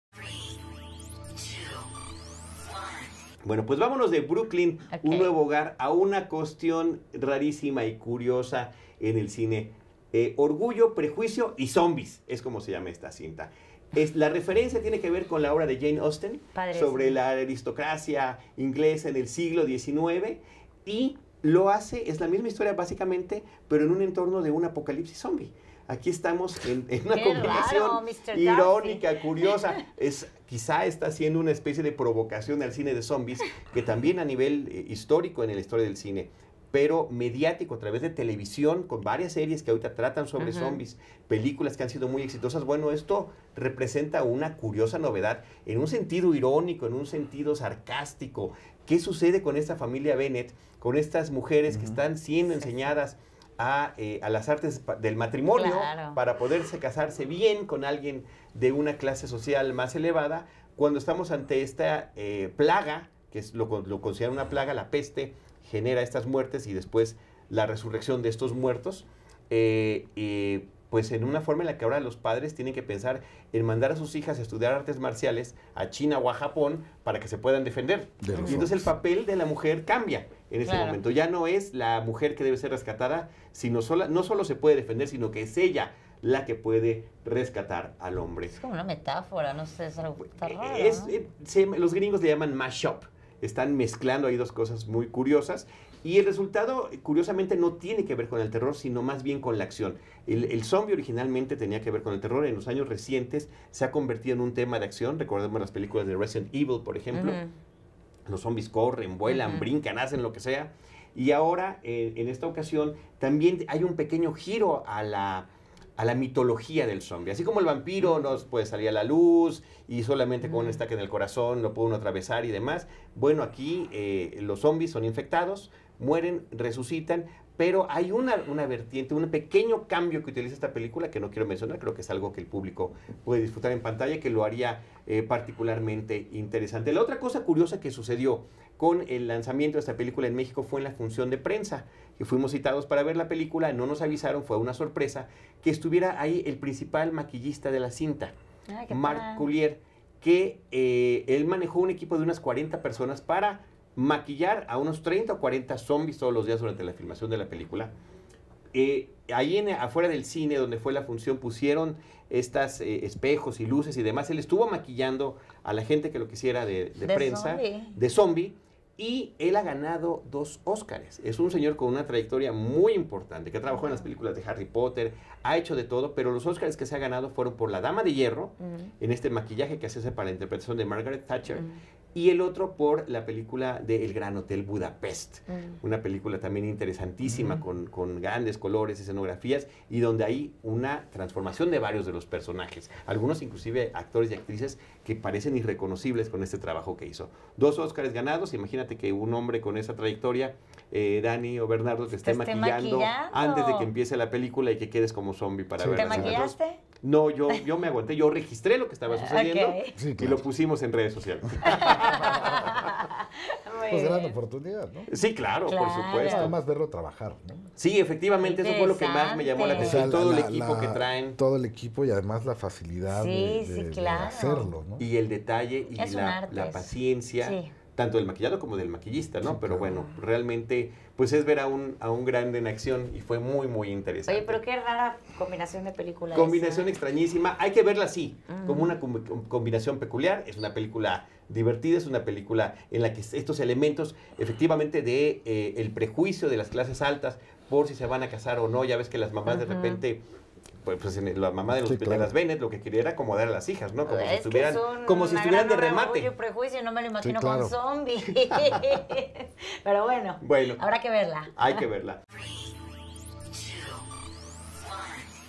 Orgullo, Prejuicio y Zombies - Review
Desde Efekto TV con Bernardo Lartigue y Daniela Carvallo, Carlos Del Río conversa acerca de la película ORGULLO, PREJUICIO Y ZOMBIES (Pride+Rejudice+Zombies, 2016).

El filme es dirigido por Burr Steers y protagonizado por Lily James, Sam Riley, Jack Huston, Bella Heathcote, Douglas Booth, Charles Dance, Lena Headey y Matt Smith.

Transmitido en vivo el 5 de Febrero de 2016 por el Canal EFEKTO TV (125 de izzi, 234 de SKY y 163 de Total Play) y Radio Capital (830 AM).